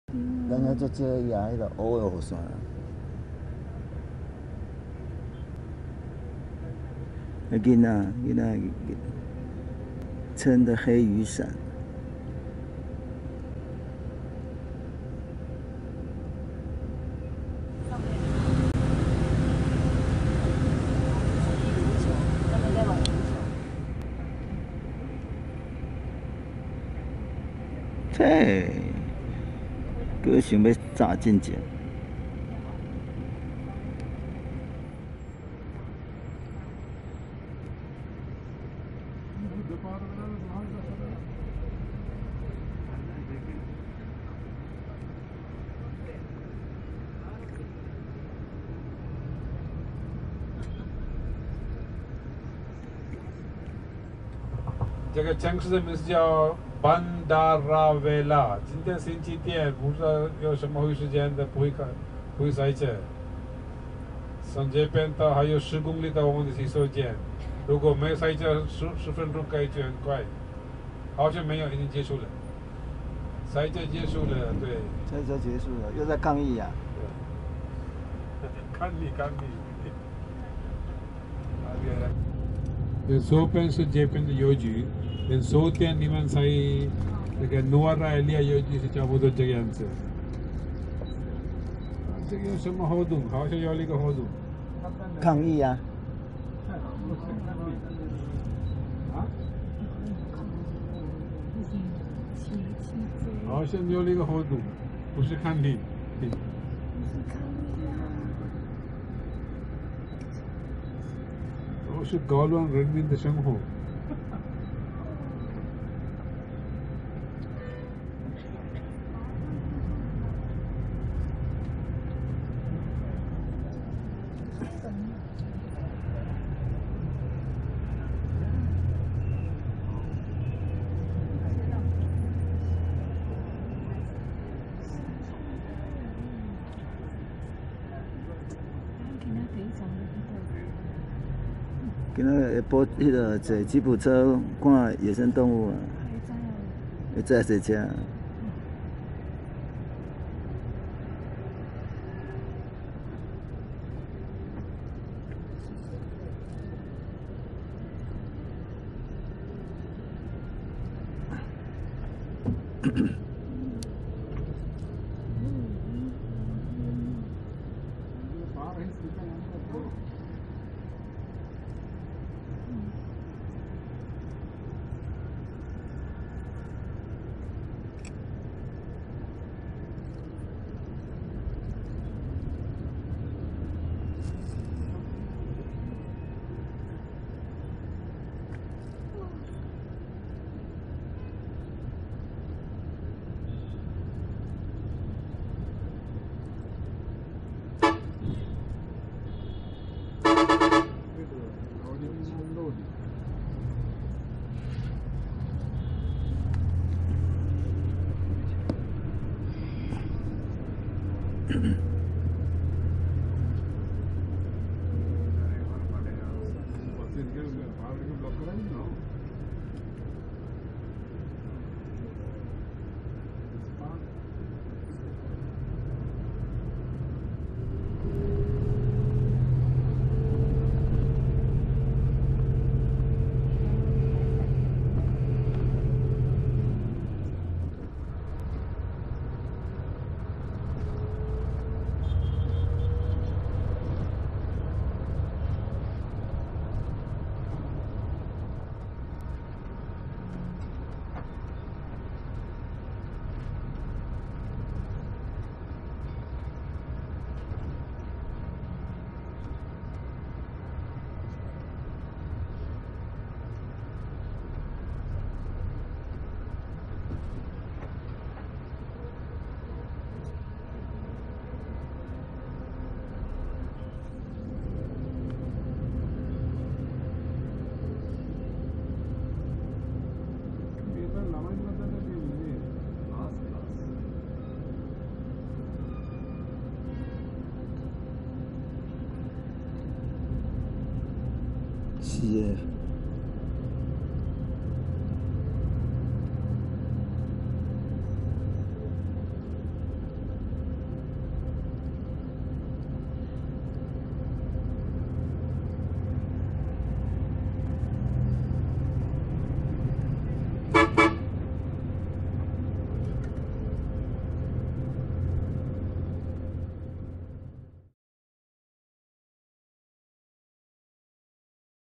斯里蘭卡往蒂瑟默哈拉默路途Bandarawela班德勒韋勒城鎮 On the way to Tissamaharama (Sri Lanka)
班德勒韋勒城鎮Bandarawela是個生機勃勃的貿易小鎮,有極其好的地理位置供遊客玩遍周邊景點.這裡氣候宜人,是養老的好去處,每個星期天早上,都會有集市,小商販們叫賣新鮮的蔬菜水果,特別熱鬧.